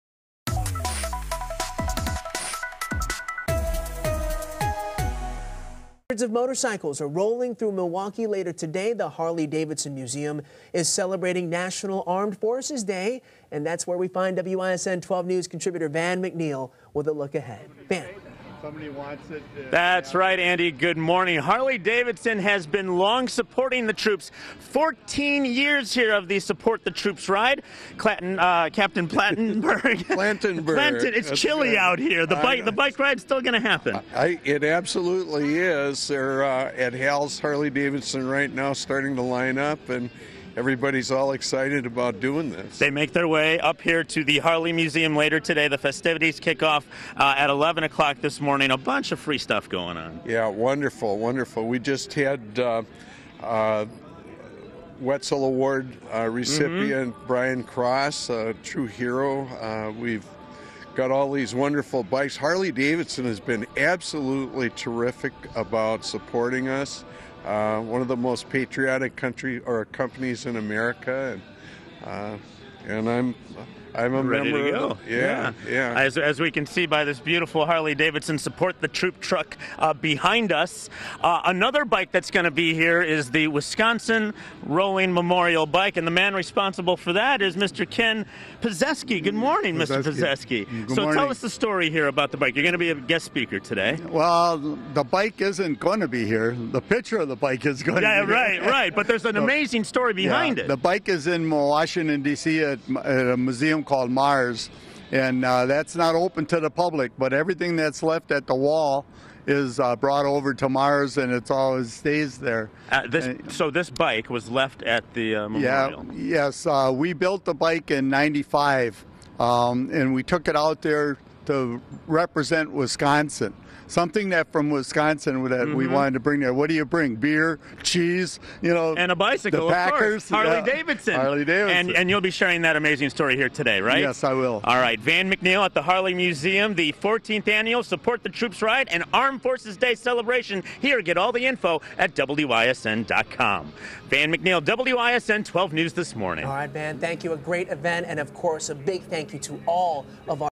Harley-Davidson Armed Forces Day Celebration
The 14th Annual Support the Troops Ride from Hal's in New Berlin to the Harley-Davidson Museum. A special Armed Forces ceremony will take place after the ride.